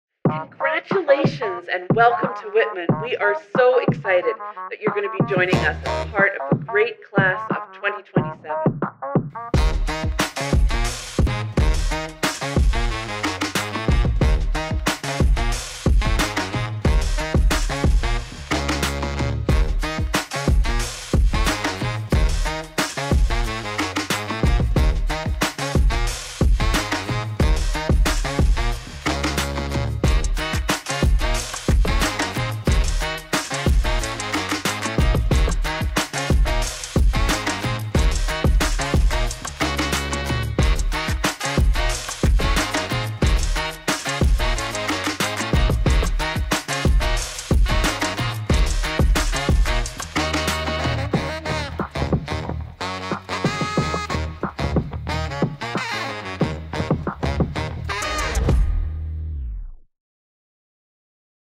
Welcome to Whitman College's Class of 2027!
💙🌟💙 We're super excited to welcome our Class of 2027—we can't wait to see you all on campus in the fall of 2023. Thanks to everyone who visited with your families for a Spring Into Whitman event.